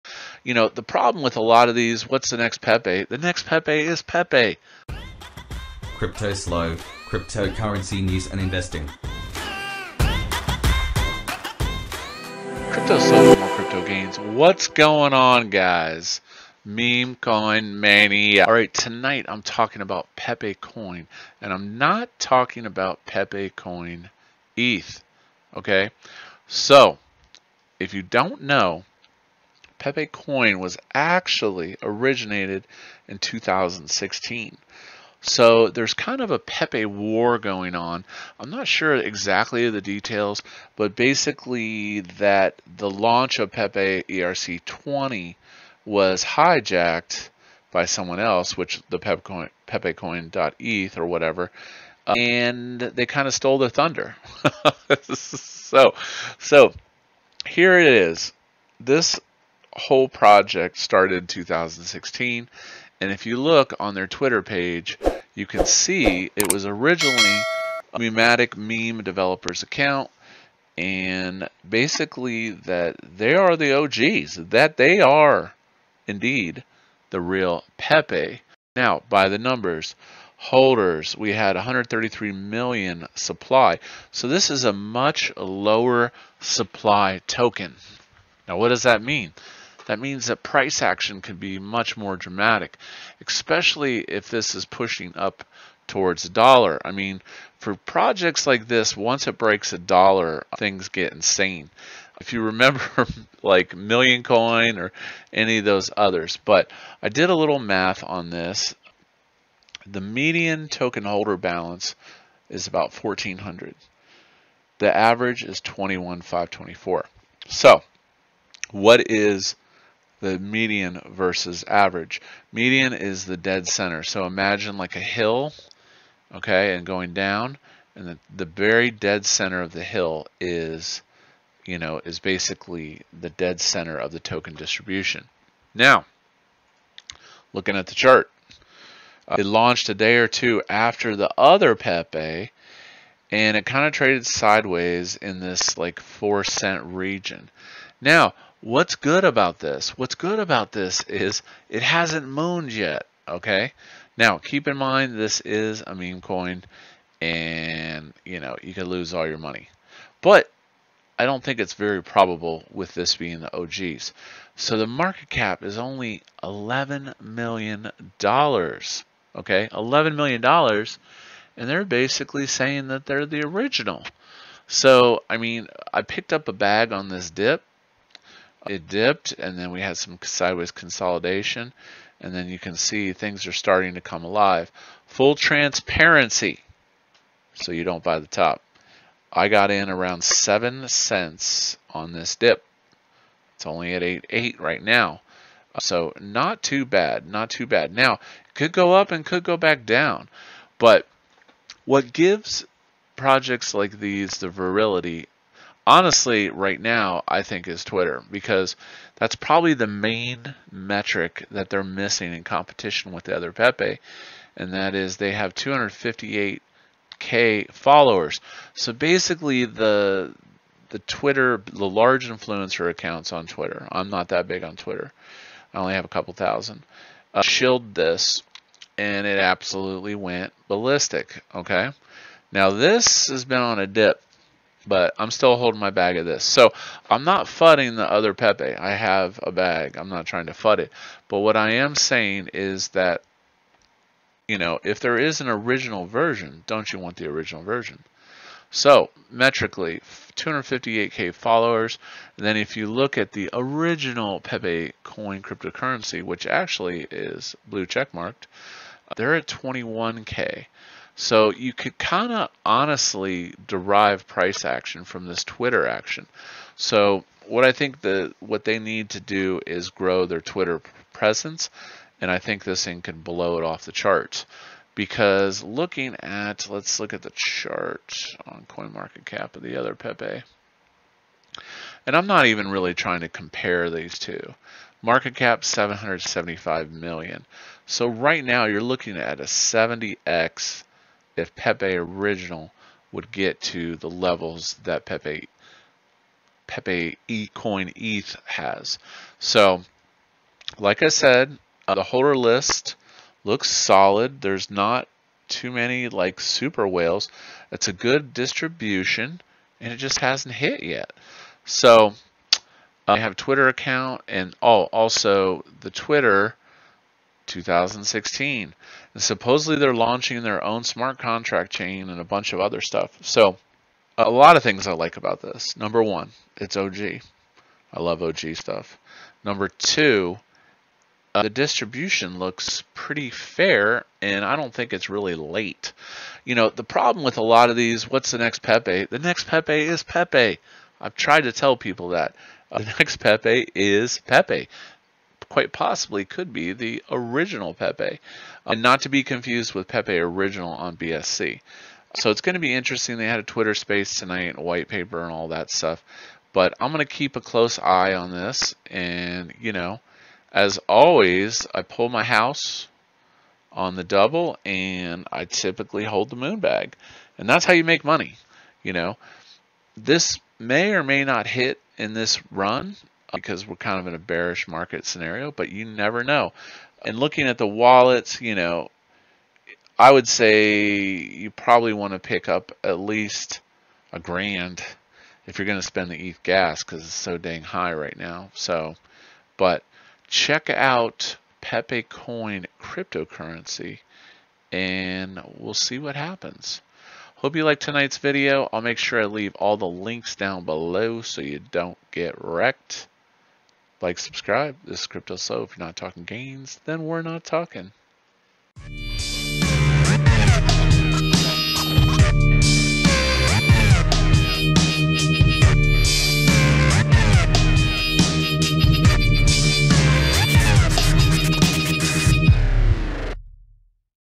The Real Pepecoins Will Mint Millionaires
In tonight's video I will talk about the ongoing war between PepeCoin Cryptocurrency and PepeCoinEth.

** LINKS ** DYOR!!! NFA!!